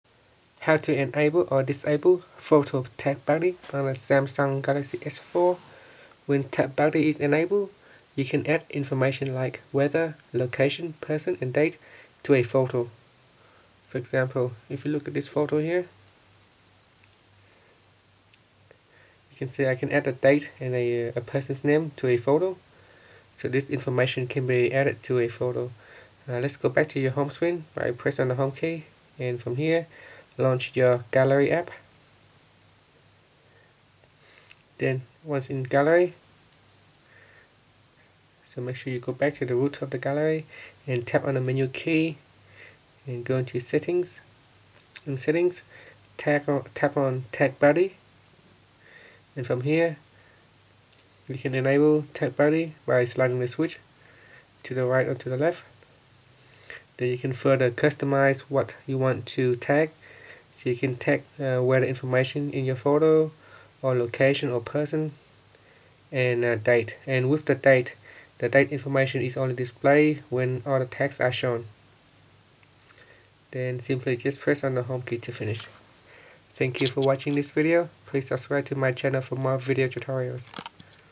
Samsung Galaxy S4: How to Enable/Disable Photo Tag Buddy
Learn how you can enable or disable photo tag buddy on the Samsung Galaxy S4.

Photo tag buddy allow you to add information such as people, weather, location or date to the photo.